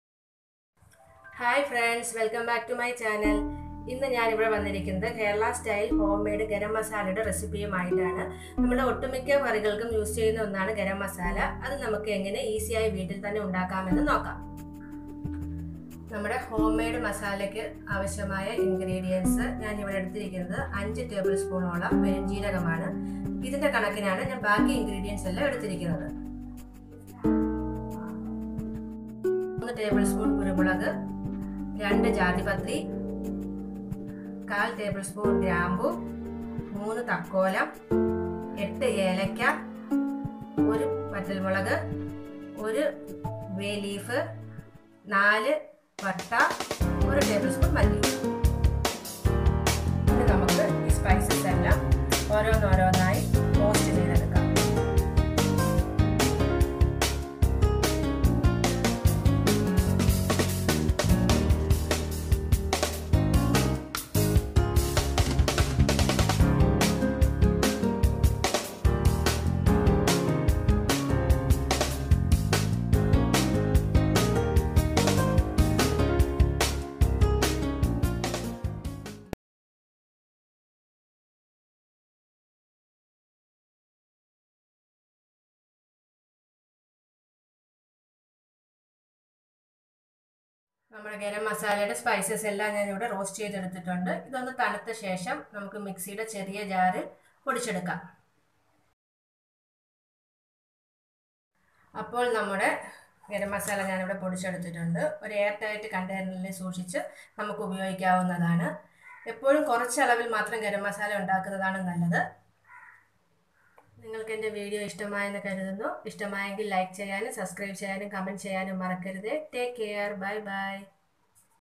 GARAM MASALA RECIPE/ഇങ്ങനെ ചെയ്താൽ ഗരം മസാല സൂപ്പറാകും/Home made Garam Masala
ഇനി വീട്ടിൽ തന്നെ ഗരം മസാല ഉണ്ടാക്കിയെടുക്കാം. വെറുതെ കടയിലെ വാങ്ങി ആരോഗ്യം നശിപ്പിക്കണ്ട

INGREDIENTS REQUIRED :
Fennel seed : 5 Table spoon
Cinnamon : 4
Star anise (thakkolam) : 3
Pepper ; 3 tsp
Long cloves : 1/2 tsp
Coriander : 1 tsp
Cardamom :  8 nos
Bay leafs : 3 nos.
Myristica : 3 (jathi patri)
Red chilly : 1
Method : Dry roast in a pan each one separately and grind it well in mixer. simple!!

Please share your suggestions, comments and keep watch my channel to enjoy more varieties and lot

Resmi R Pillai